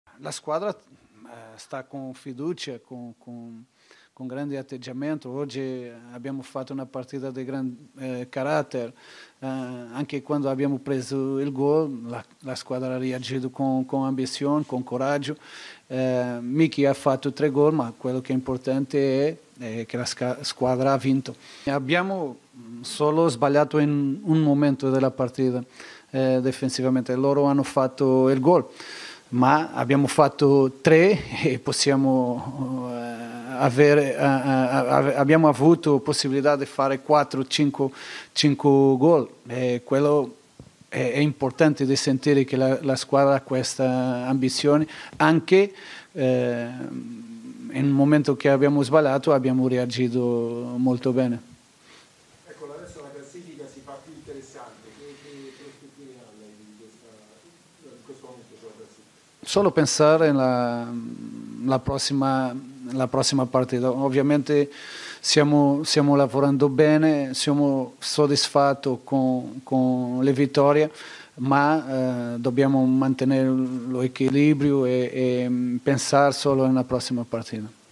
Genoa-Roma, Fonseca: "Vittoria di oggi è prova di carattere"
Allenatore in conferenza: "Ora concentrati su prossima partita"

SPORT (roma). "La squadra sta con un grande atteggiamento. Oggi abbiamo fatto una partita di grande carattere, anche quando abbiamo preso un goal la squadra ha reagito con coraggio e ambizione. Abbiamo sbagliato solo in un momento della partita, ma noi abbiamo fatto 3 goal e avevamo la possibilità di farne altri. Ora siamo concentrati sulla prossima partita." Così Paulo Fonseca, allenatore della Roma, commentando la partita Genoa-Roma in conferenza stampa. (Teresa Ciliberto/alanews)